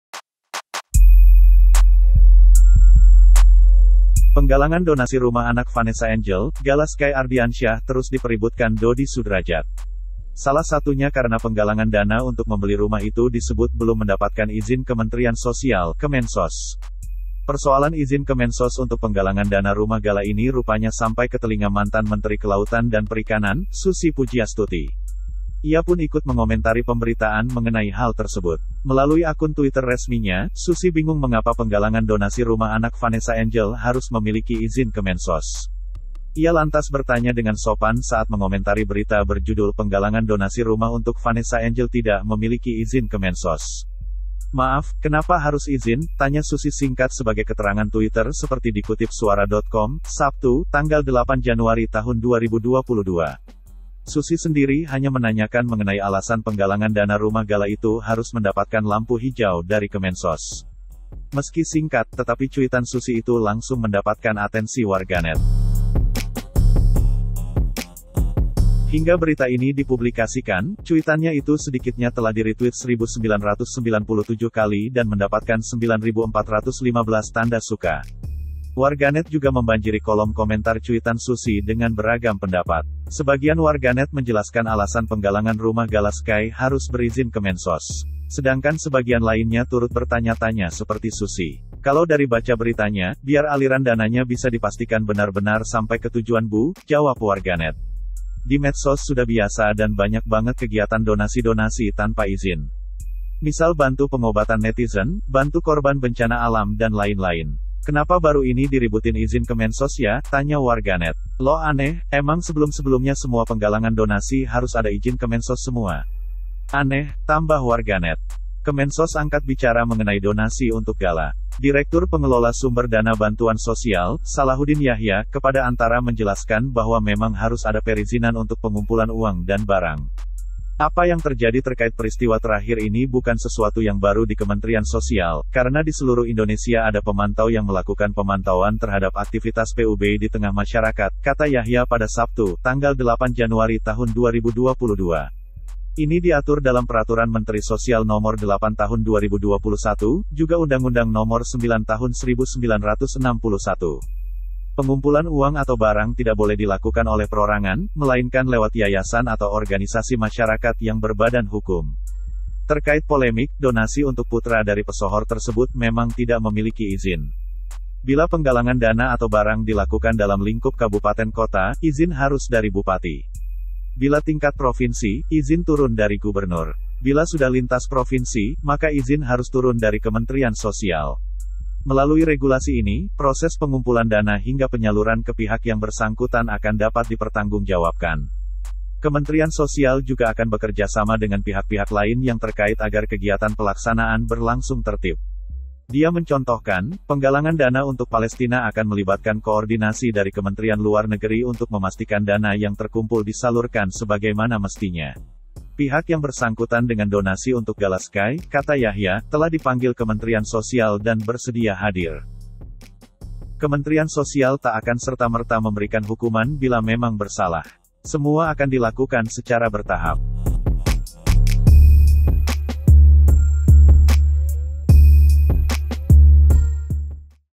Susi Pudjiastuti Tanya Mengapa Donasi Rumah Gala Sky Harus Izin Kemensos
Penggalangan donasi rumah anak Vanessa Angel, Gala Sky Ardiansyah terus dipeributkan Doddy Sudrajat. Salah satunya karena penggalangan dana untuk membeli rumah itu disebut belum mendapatkan izin Kementerian Sosial (Kemensos). Persoalan izin Kemensos untuk penggalangan dana rumah Gala ini rupanya sampai ke telinga Mantan Menteri Kelautan dan Perikanan, Susi Pudjiastuti. Ia pun ikut mengomentari pemberitaan mengenai hal tersebut. Berikut selengkapnya.